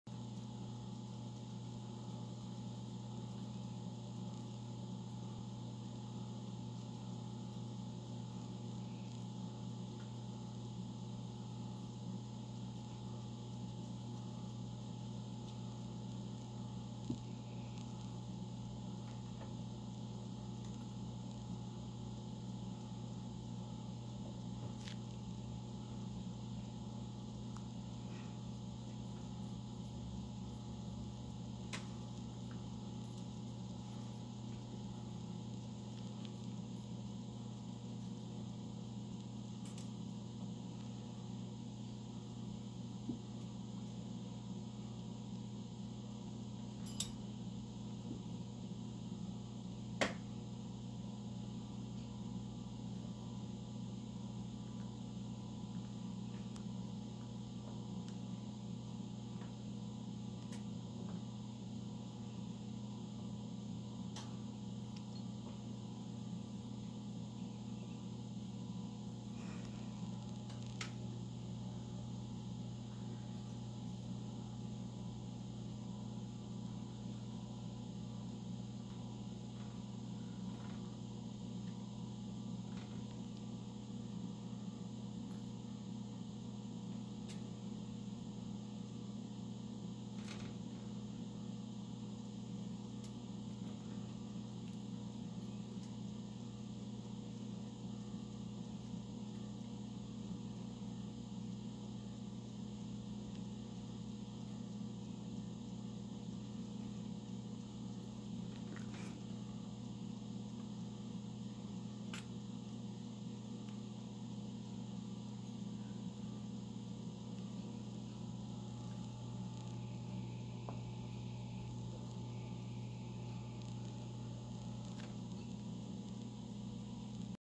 Breakfast time!
Here are Impa, One and Science eating some berries and peppers for breakfast! I could sit here all day and listen to them eat all day and watch them with their grabby hands. I love them so much :')